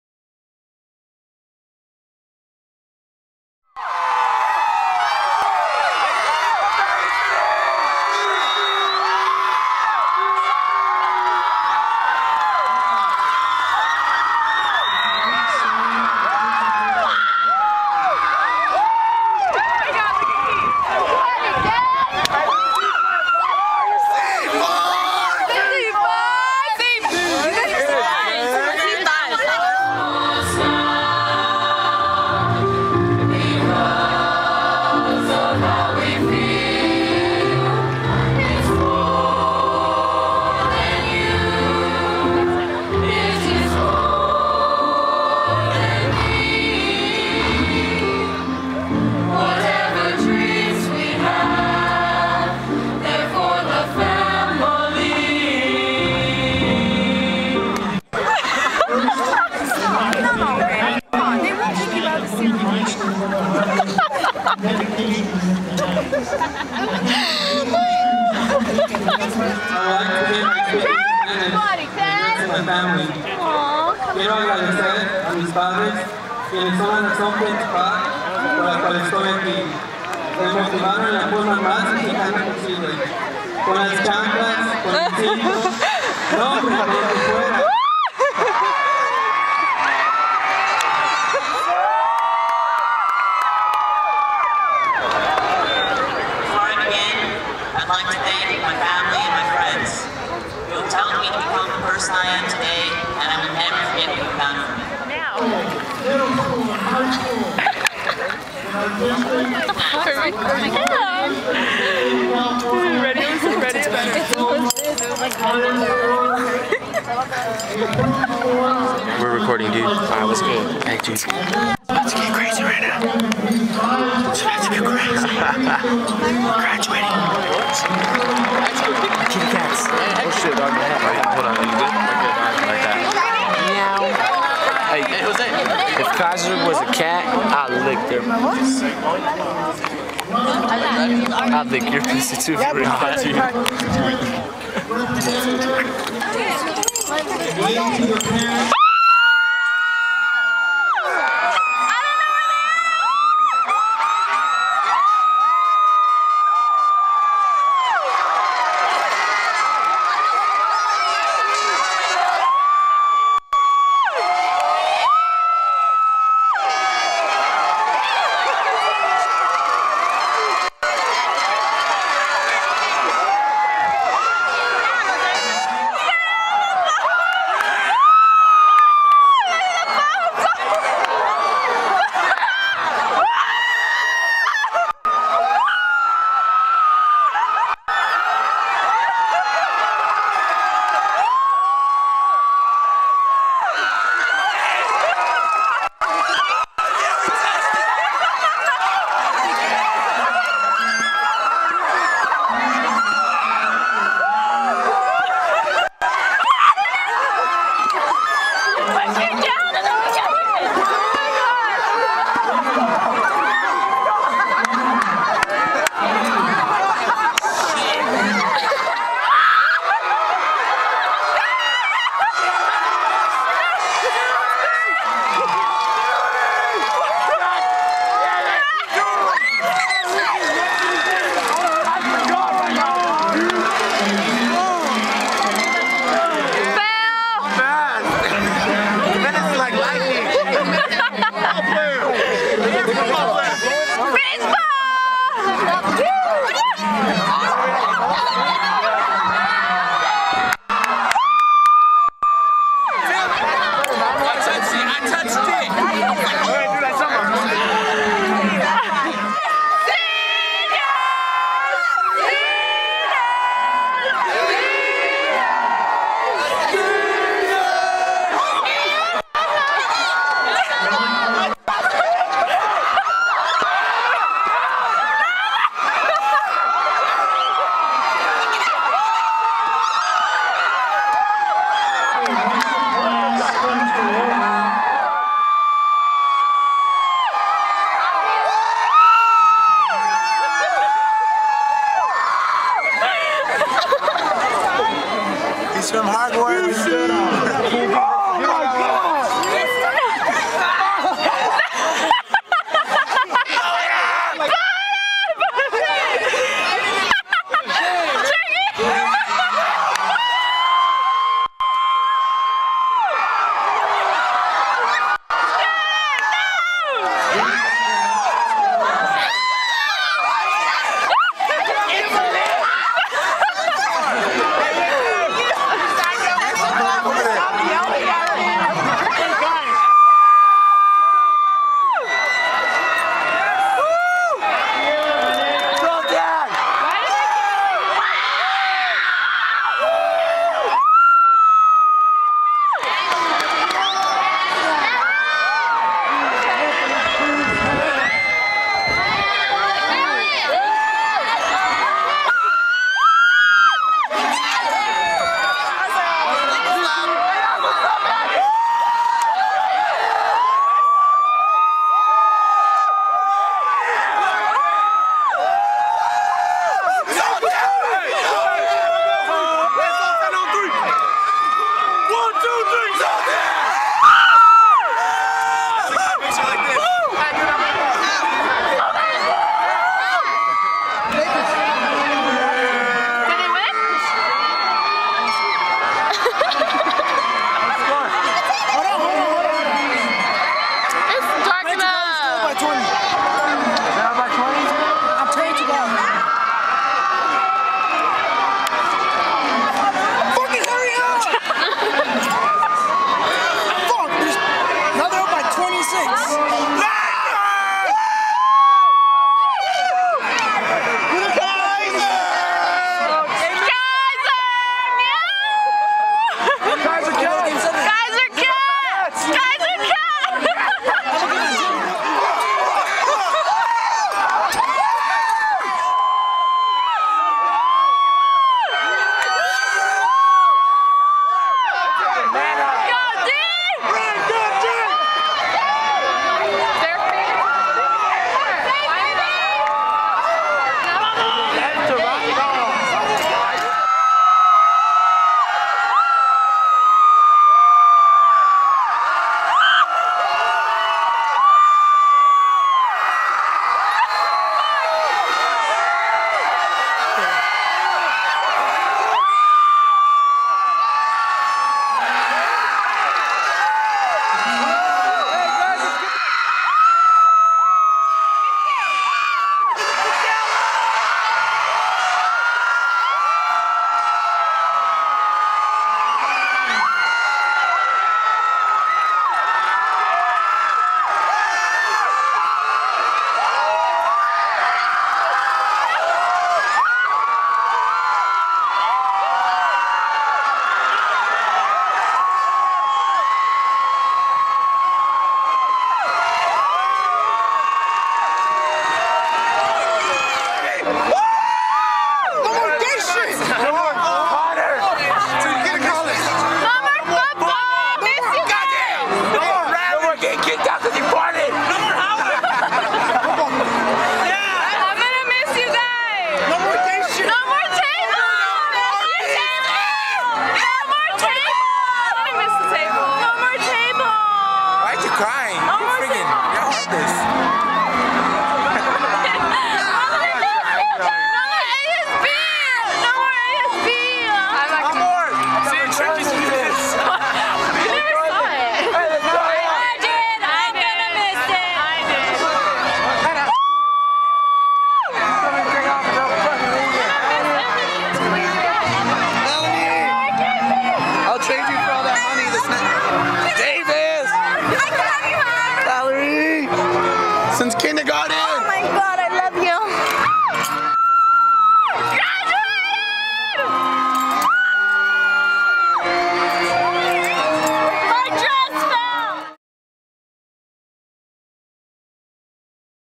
BHS 2010 GRADUATION
ahhhh, imma miss all you guys :/
this video is pretty much just the screaming ahahha, but BLOW UP DOLL!!! hahahaha